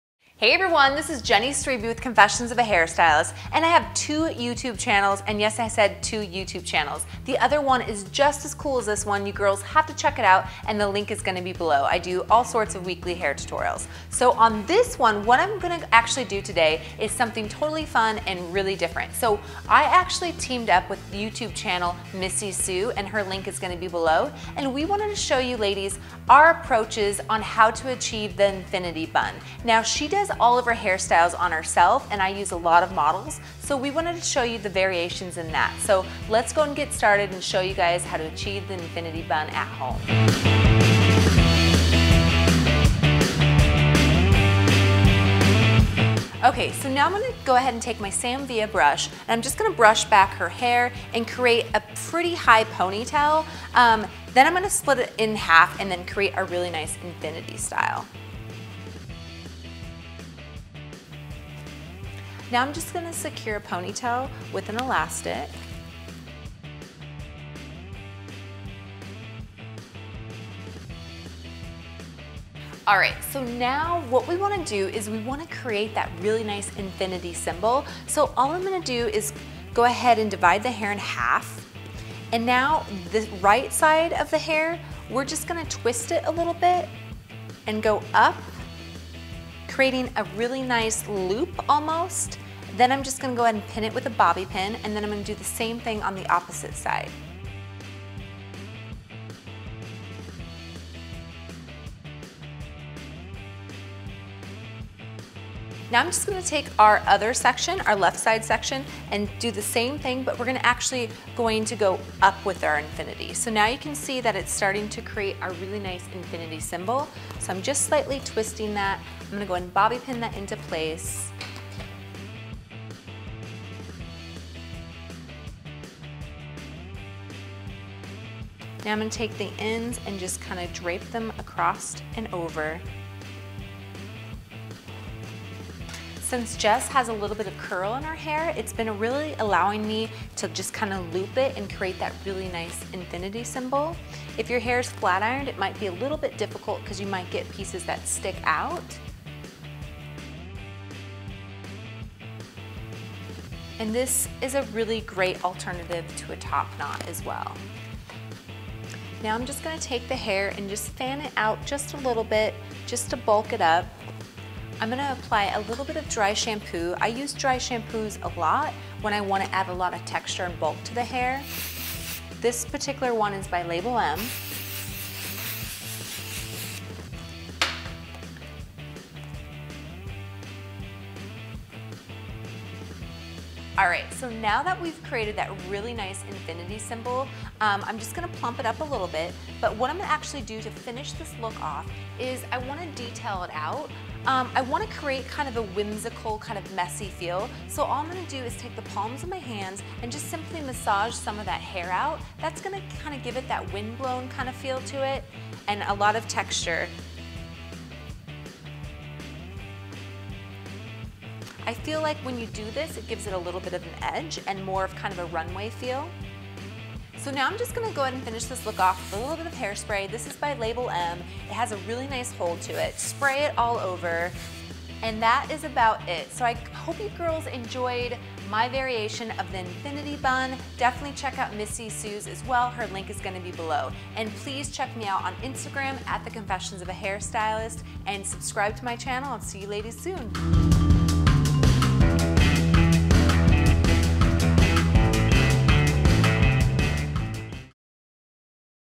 Infinity Knot Bun
Infinity Knot Bun is easy and elegant. Impress all of your friends with this hairstyle!

Quick tips for busy women and moms who want to learn more about cooking, beauty, style, DIY, fitness and parenting